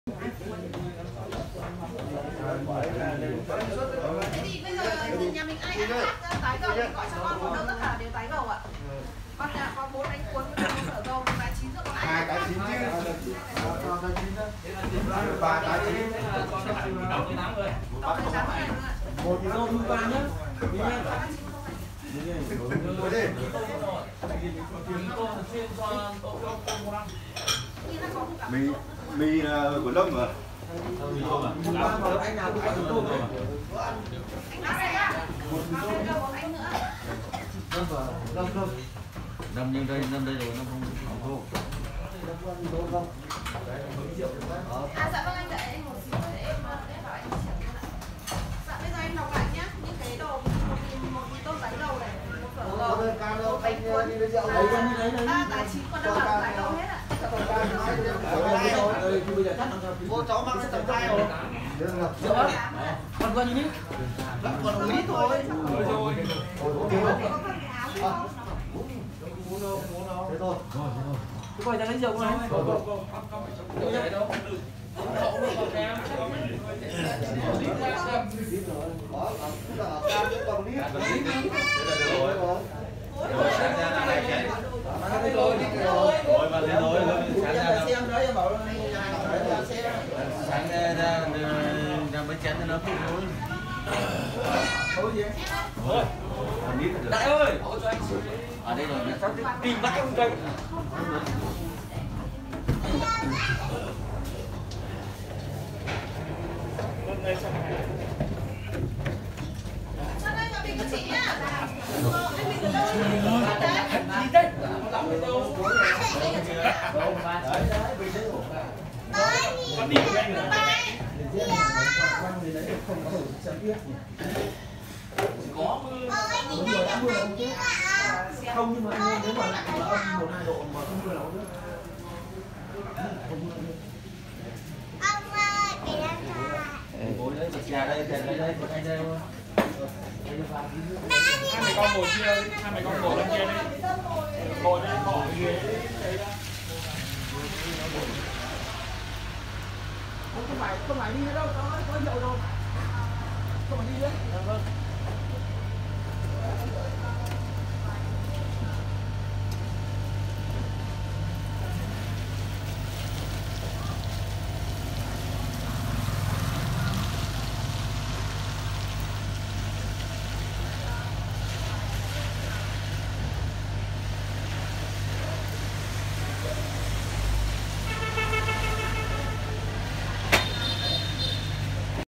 Sapa 0 độ quán phở hút khách nhờ đốt than sưởi ấm.
Sa pa nhiệt độ xuống âm, du khách đổ lên đây chờ tuyết xuống.